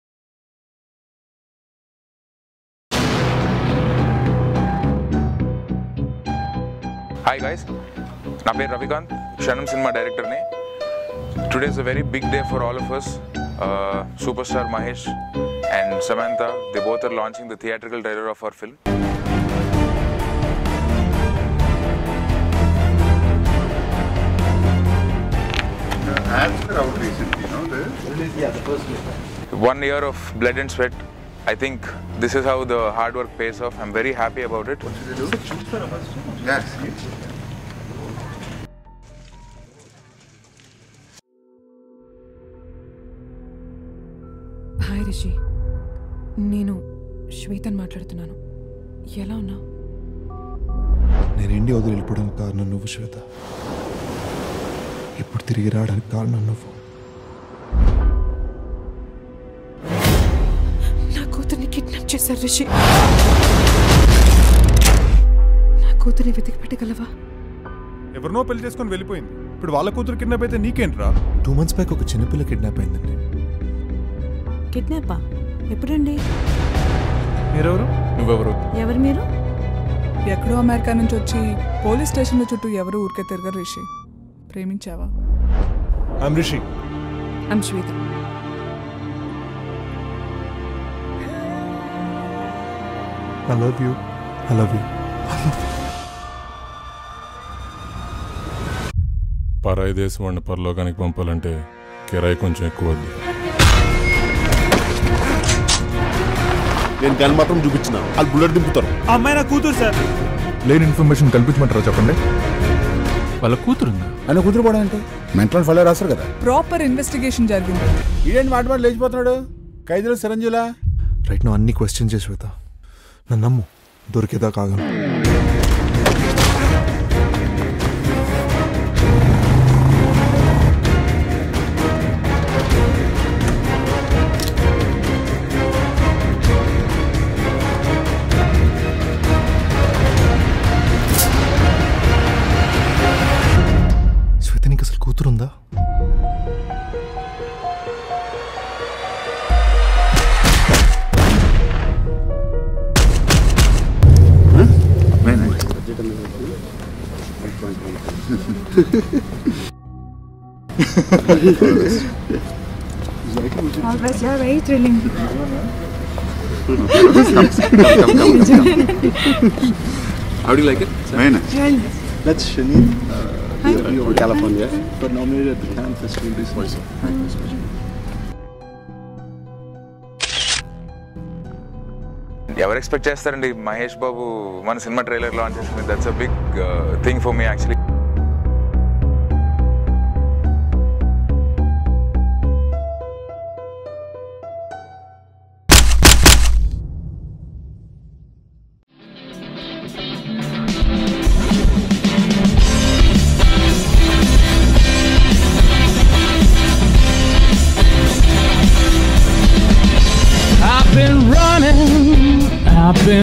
Kshanam Trailer Launch by Mahesh Babu and Samantha Ruth Prabhu || Adivi Sesh, Adah Sharma, Anasuya
Watch Kshanam Trailer Launch by Super Star Mahesh Babu and Samantha Ruth Prabhu and PVP

Starring : Adivi Sesh, Adah Sharma, Anasuya Bharadwaj
Producer : Param V. Potluri
Director : Ravi Kanth Perepu